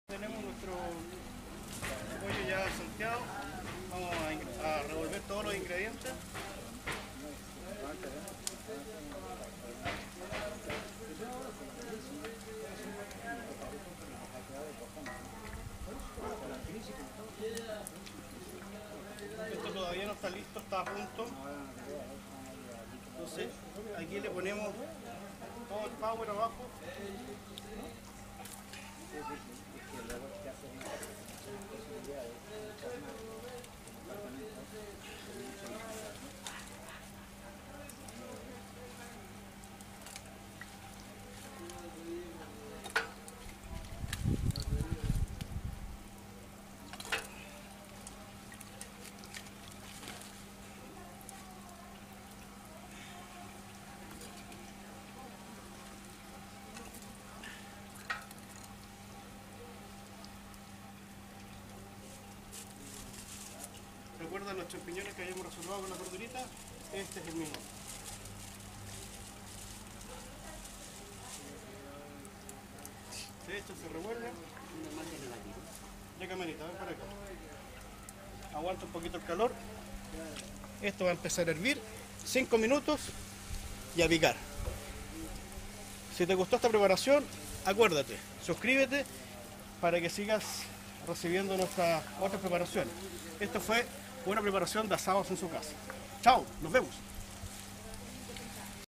Asado al disco para picar 3/3
Preparacion 3ra parte y final de un asado al disco para picar.